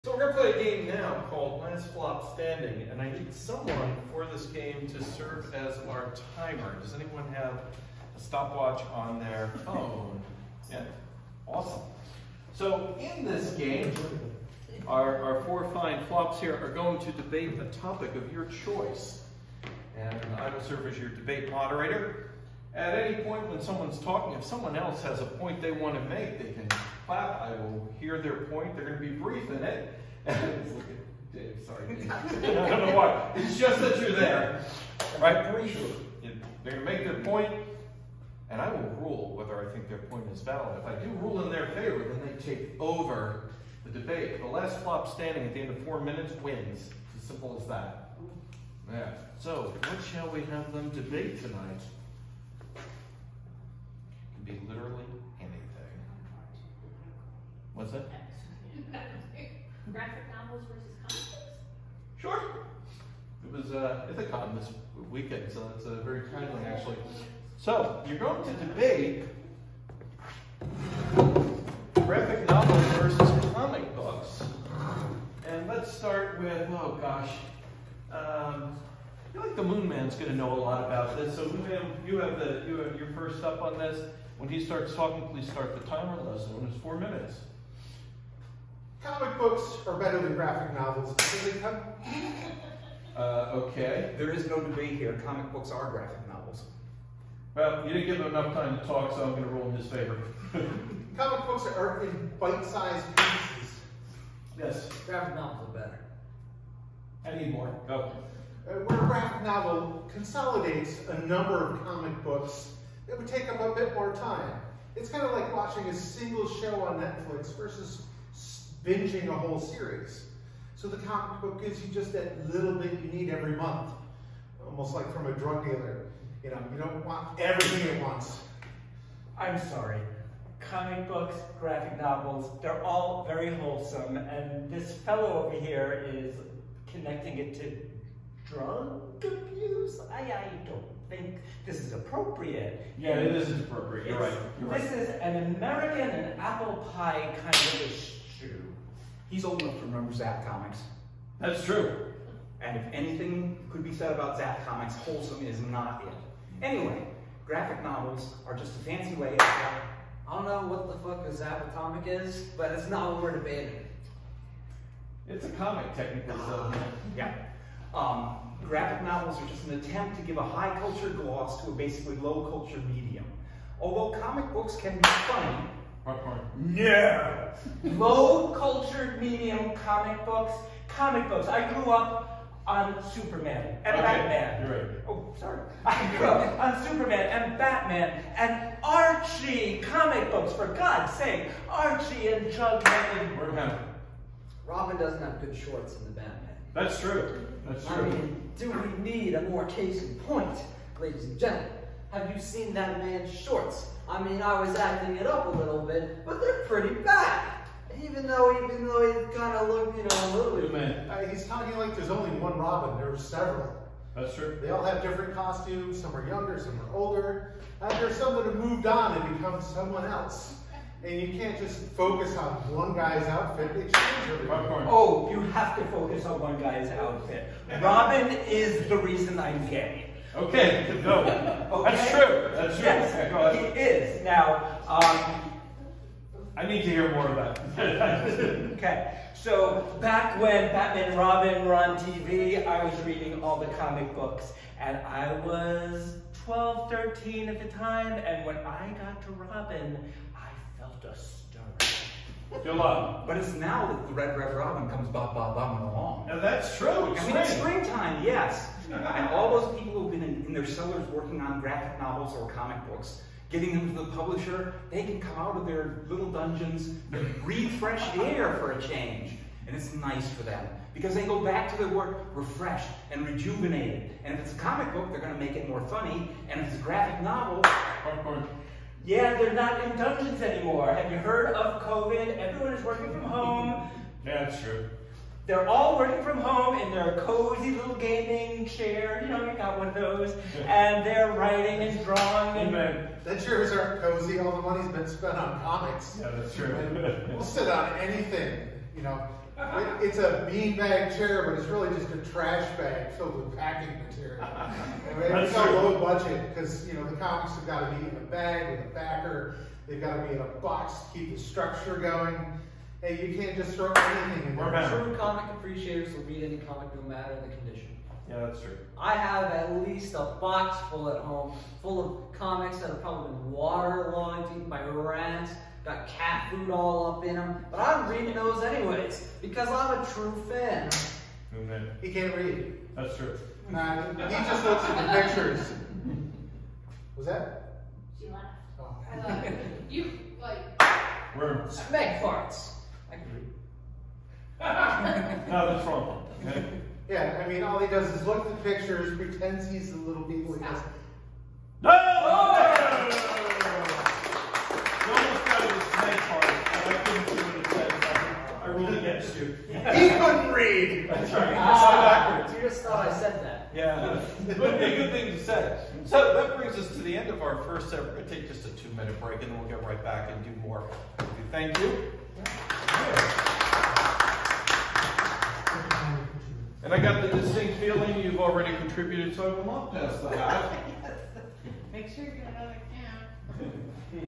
08 Last FLOP Standing 2023 04 25 Ake Gallery
The cast debate a topic but at the end of 4 minutes the one speaking is the only one who can win.